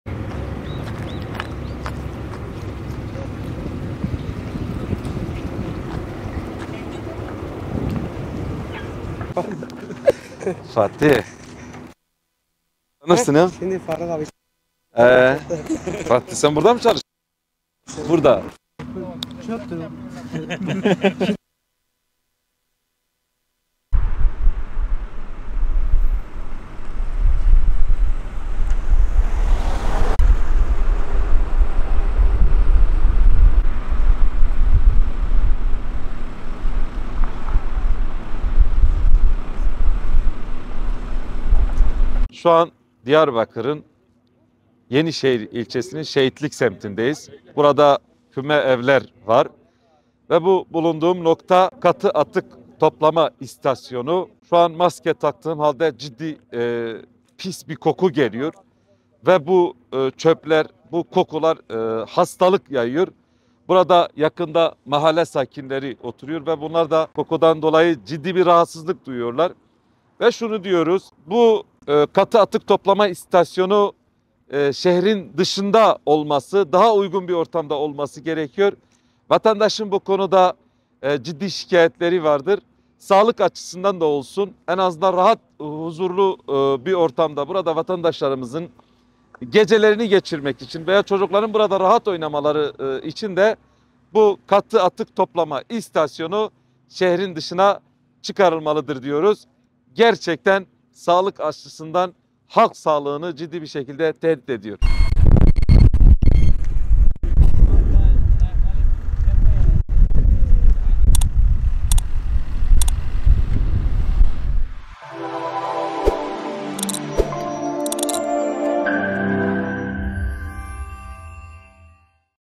HÜDA PAR'ın Diyarbakır adayı Dinç: Katı atık istasyonu, şehrin dışına çıkarılmalı
HÜDA PAR Diyarbakır Büyükşehir Belediye Başkan Adayı Faruk Dinç, Yenişehir ilçesinde bulunan katı atık arıtma istasyonunun, çevreyi rahatsızlık verdiği için merkezden uzak bir yere götürülmesi gerektiğini belirtti.

Değerli İzleyicilerimiz;